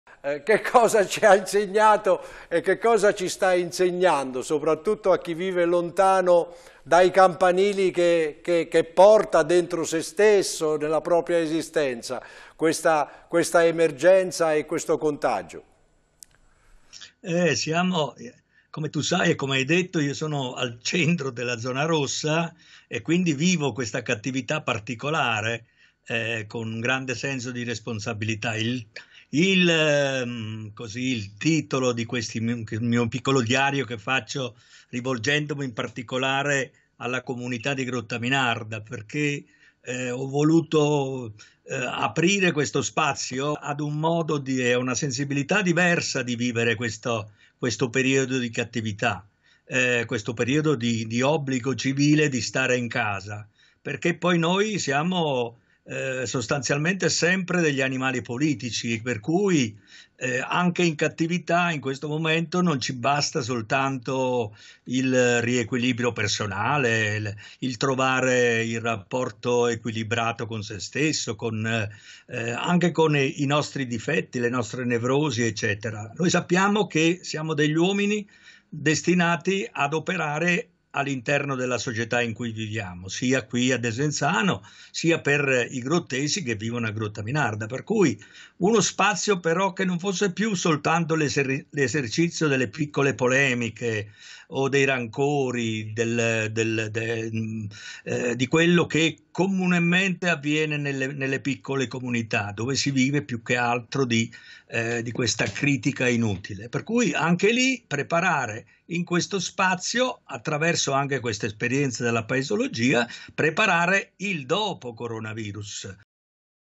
Il dopo Coronavirus. Mauro Orlando: il futuro dipende da tutti noi
Mauro Orlando, docente di filosofia e fondatore del gruppo “Grottaminarda Paesologica” che vive il Lombardia ci ha parlato delle certezze, anche politiche, venute meno in territori considerati fino a ieri un modello